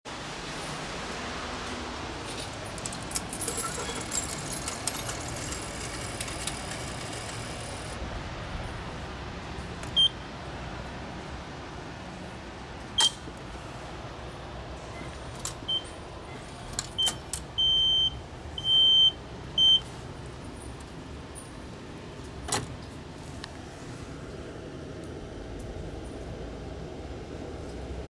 阪神電車の券売機で往復きっぷ140円区間を購入してみた 甲子園駅
2015年3月19日（木）撮影。阪神電車甲子園駅の券売機で往復きっぷ140円区間（280円）を購入する動画です。